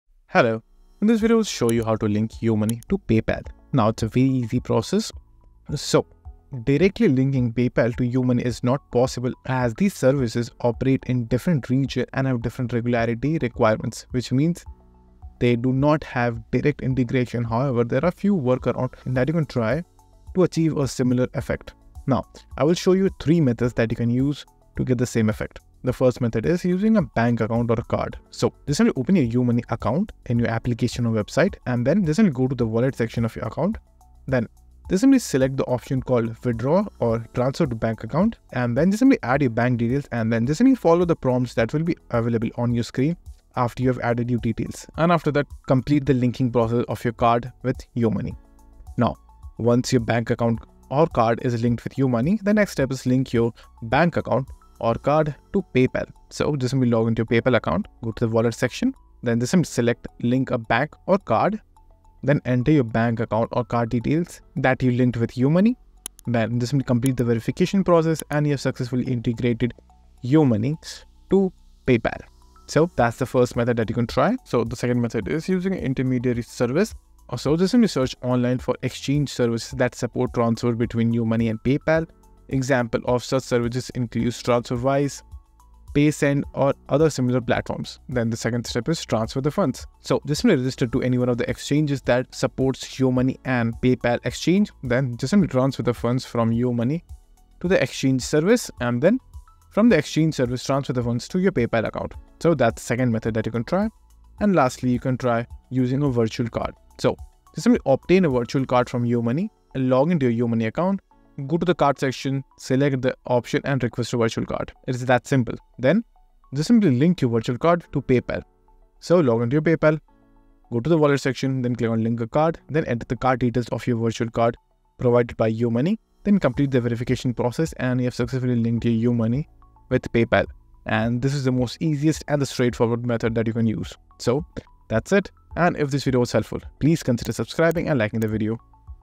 How To Link Yoomoney To Paypal - Quick Explained (2025)
Welcome, today I will show you how to connect Yoomoney to Paypal.